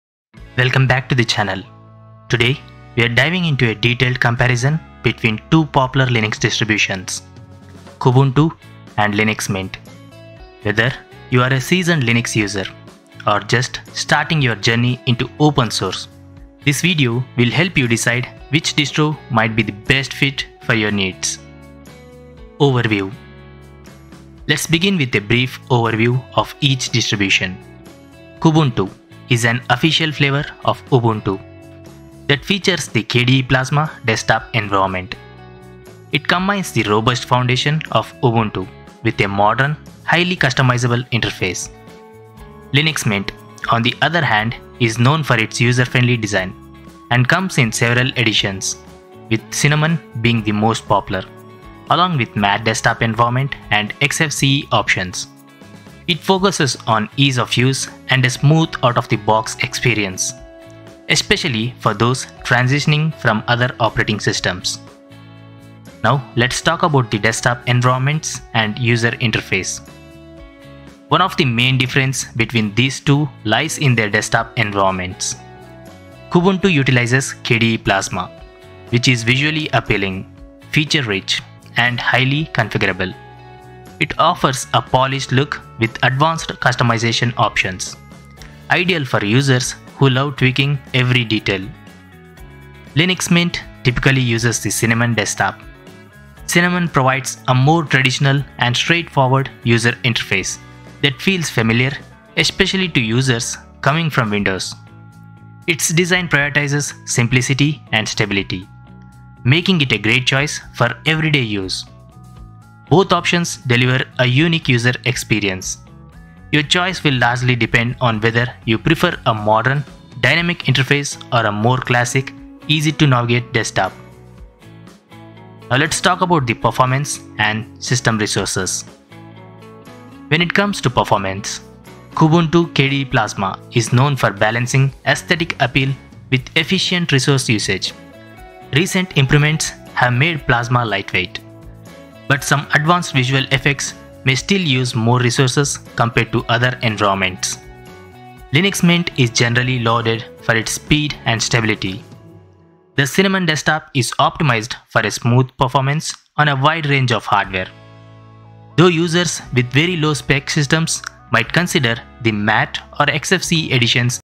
Kubuntu vs Mint: BEST Linux for YOU? 🤔
Choosing between Kubuntu and Linux Mint? 🤔 This in-depth comparison for 2025 breaks down everything you need to know to pick the perfect Linux distro! We compare desktop environments (KDE Plasma vs Cinnamon), performance, user-friendliness, software, and more.  Ideal for beginners and experienced users alike!  Learn which Ubuntu-based distro – Kubuntu or Linux Mint – is the best fit for your needs. Watch now and decide!